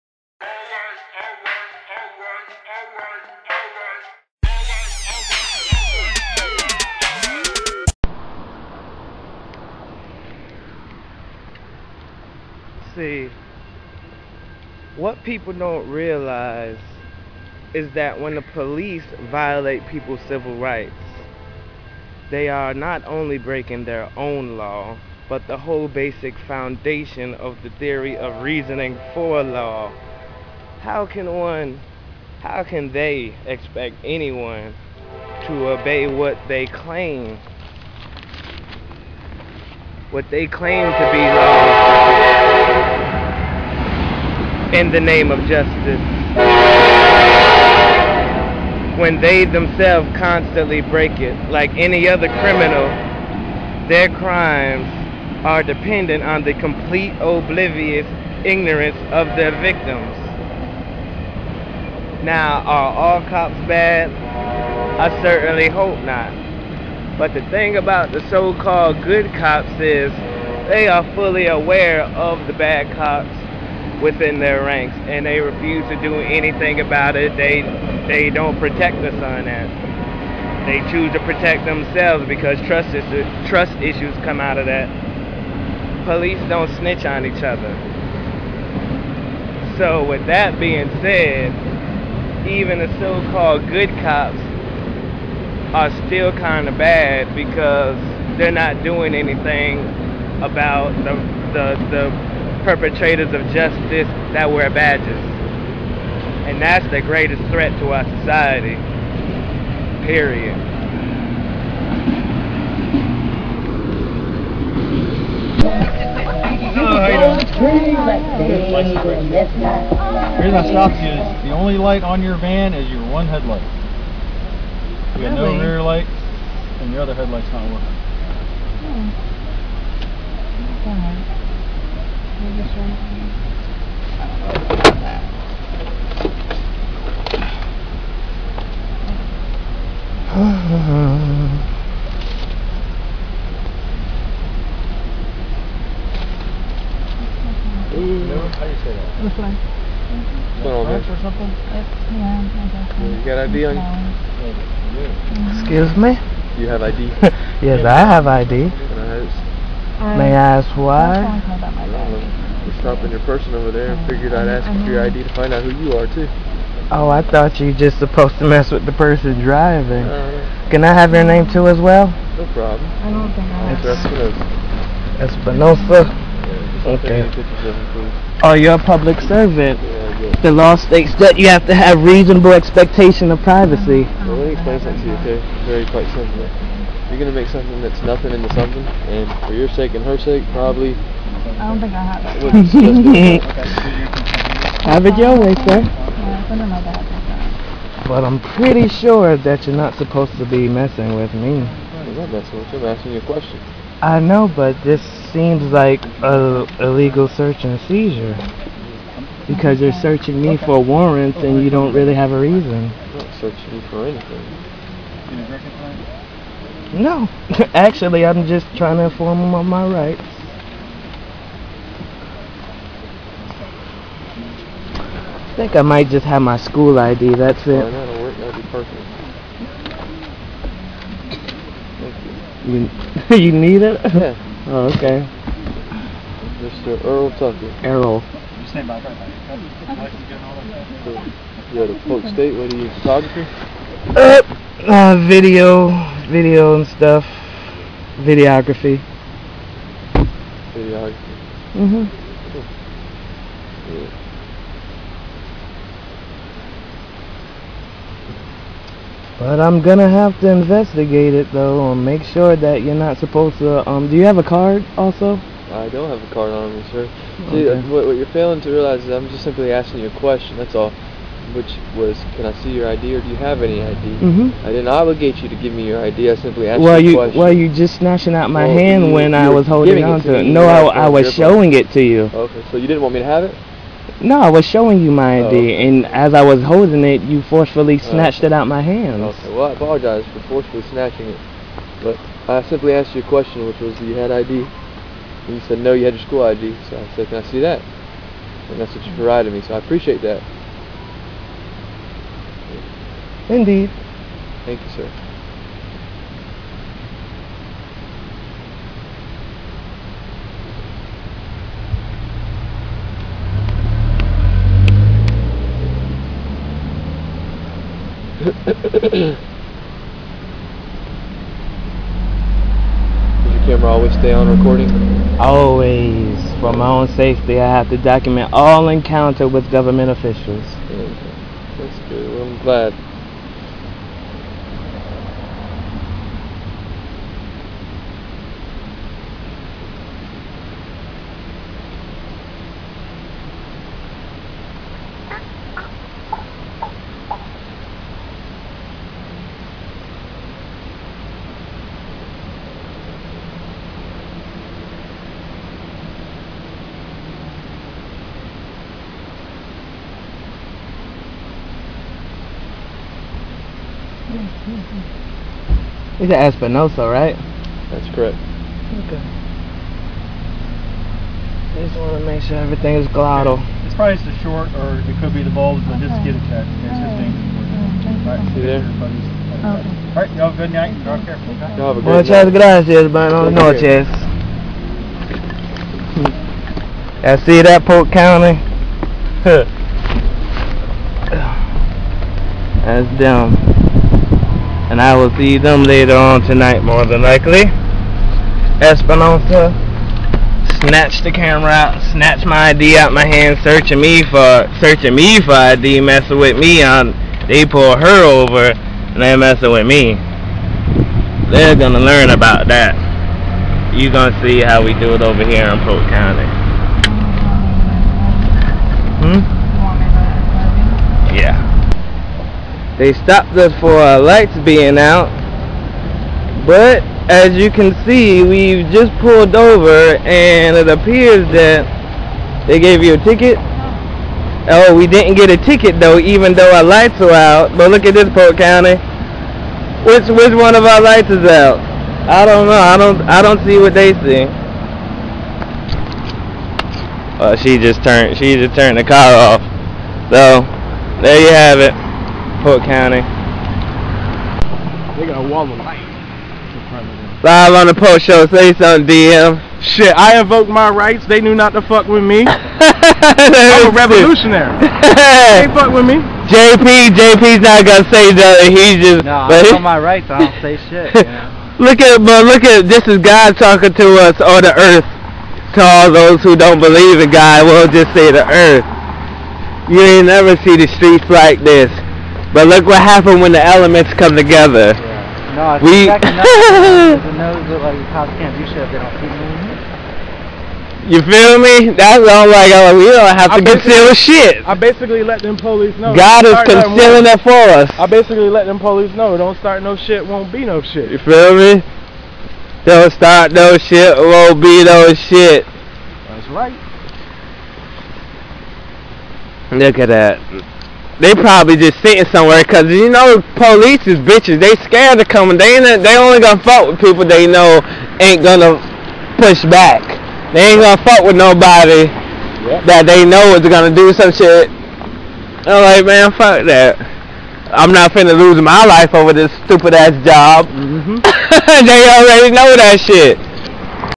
THE POLK SHOW... WESTSIDE COPWATCHERZ
GOT PULLED OVER BY THE POLICE, NOTICE HOW THE OFFICER HAD TO THREATEN ME AND MY FAMILY WHEN I ENVOKED MY RIGHTS...WHAT PPL DON'T REALIZE IS WHEN THE POLICE VIOLATE PPL'S CIVIL RIGHTS, THEY'RE BREAKING THE LAW SO HOW ARE WE SUPPOSED TO RESPECT THE LAW WHEN THEY MAKE A CONSTANT MOCKERY OF IT??? THE POLK SHOW GOTCHO BACK! CUZ "WE THE PEOPLE" ARE NOT SAFE.